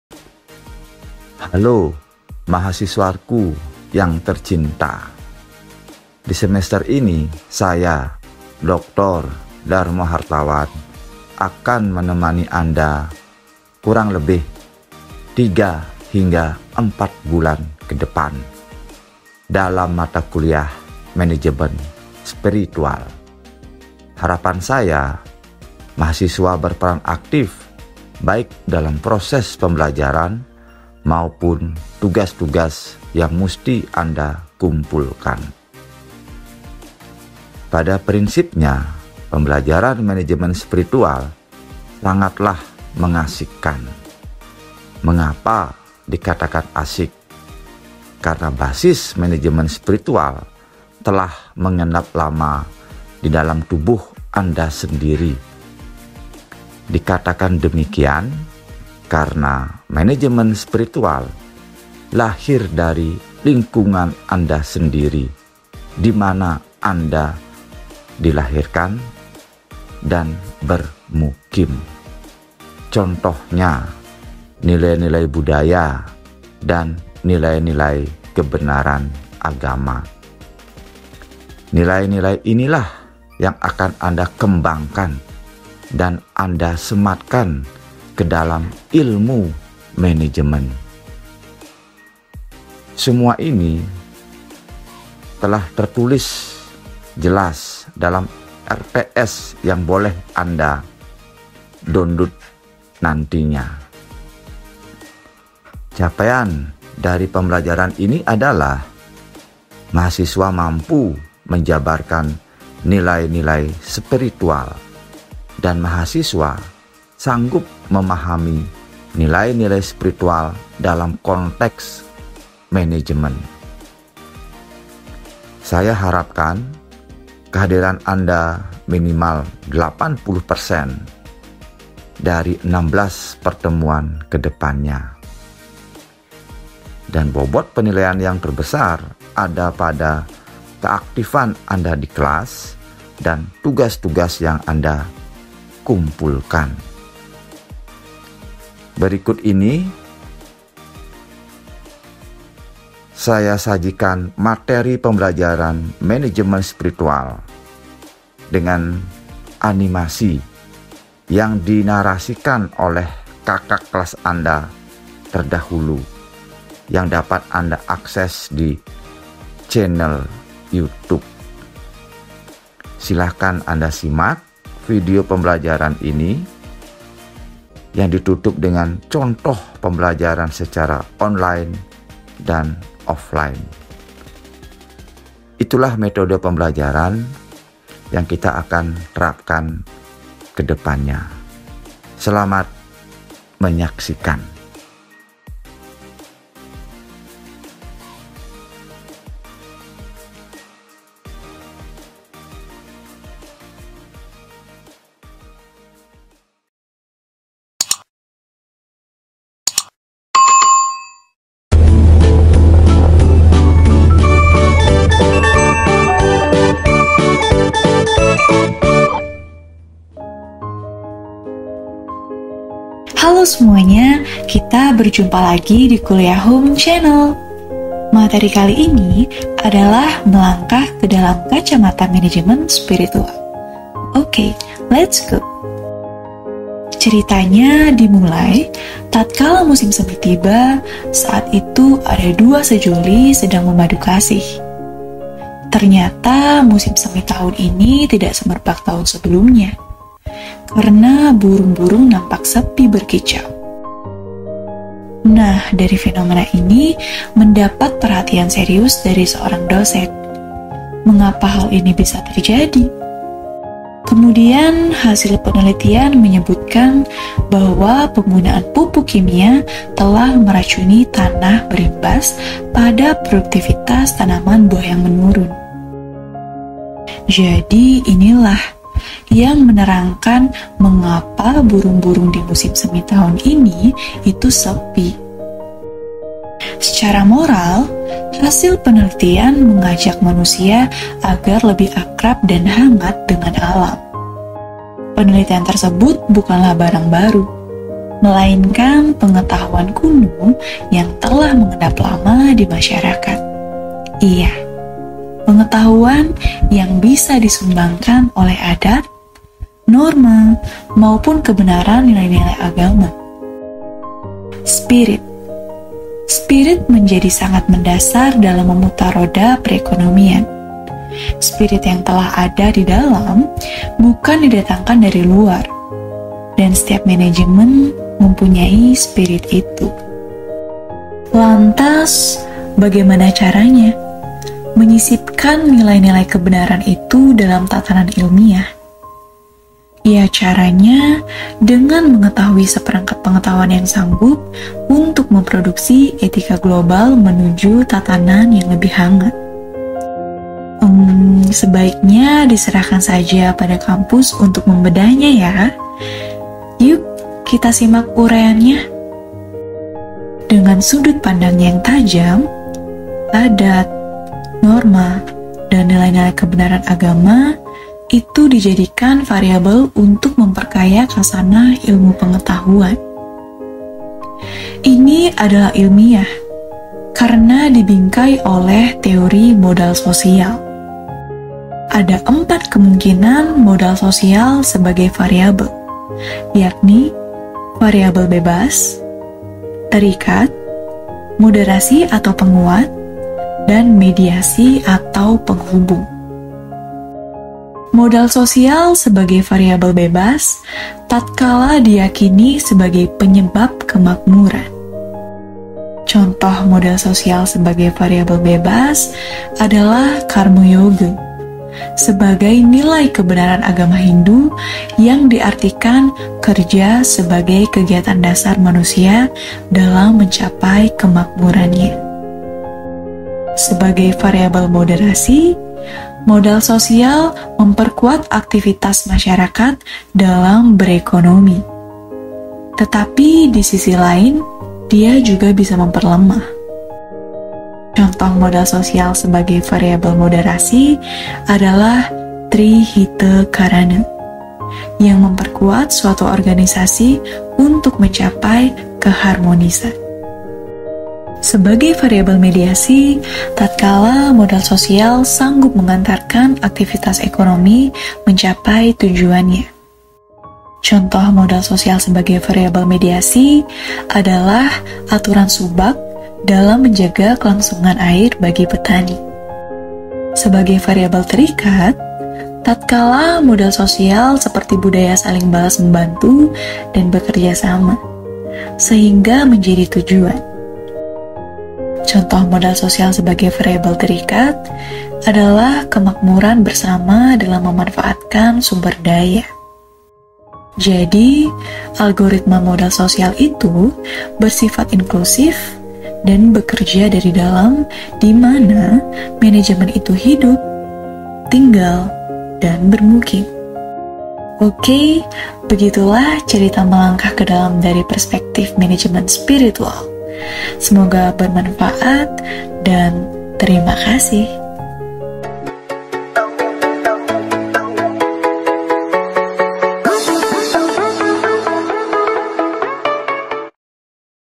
Pembelajaran Manajemen Spiritual
matakuliah ini menukik pada nilai-nilai budaya dan kebenaran agama dalam konteks manajemen